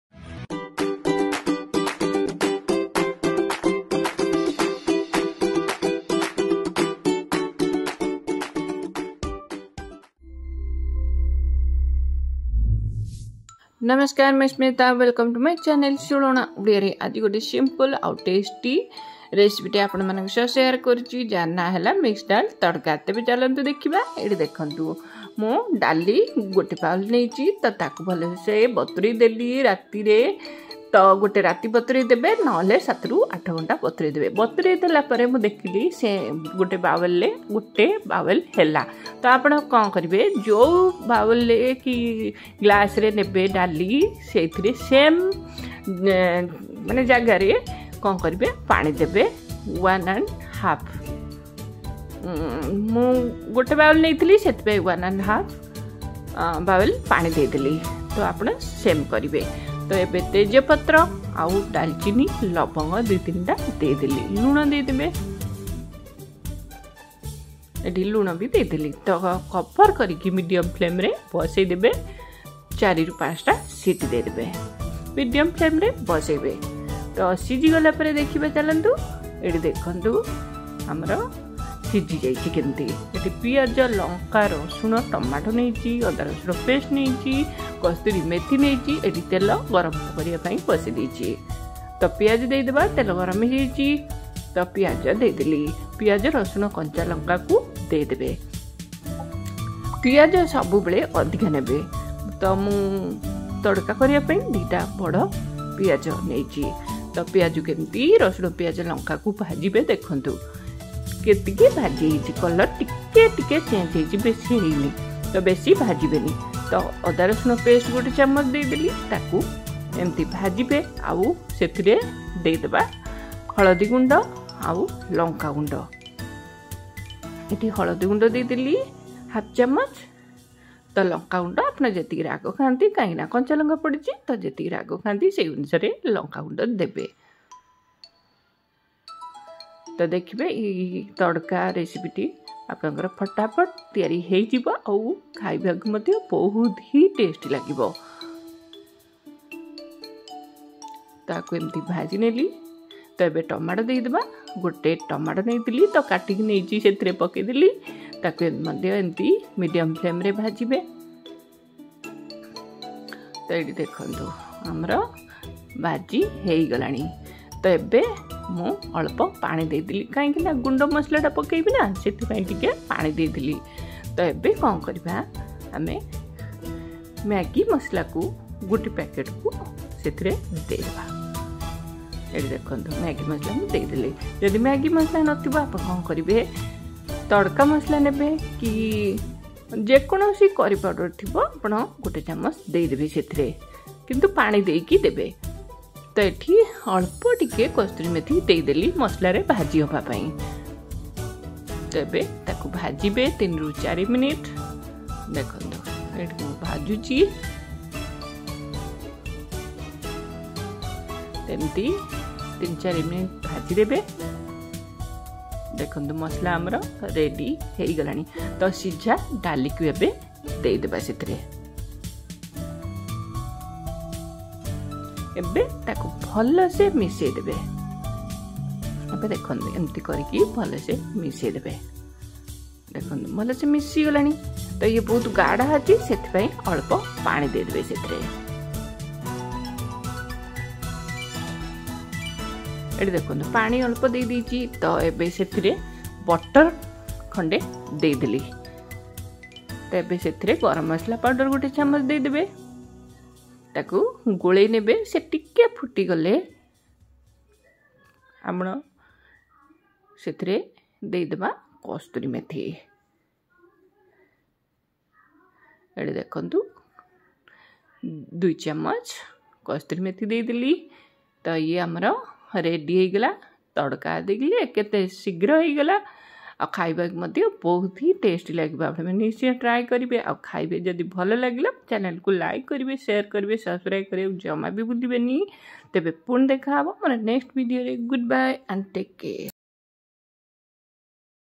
Mixed Dal Tadka Recipe/Simple Recipe/Easy To Cook
Mixed Dal Tadka#How To Make Dal Tadka or Dal Fry/16 Ana Odia by Smita

Ingredients:-
Mixdal -1 bowl
Water 1 1/2 bowl (same bowl)
Onion -2 large size
Garlic -1 tbsp/4-5 cloves
Green chillies - 5-6
Tomato - 1 larg size
Bay leaf -1
Cinnamon -1
Cloves -2-3
Maggie masala -1 pk/tadka masala/curry powder
Cooking oil - tbsp

ବହୁତ ସହଜ ଆଉ କମ ସମୟ ରେ ପ୍ରସ୍ତୁତ କରନ୍ତୁ dal tadka recipe,how to make mixed dal tadka recipe.best dal tadka recipe at home,dal fry recipe
Happy Cooking 😊
JAY JAGANNATH 🙏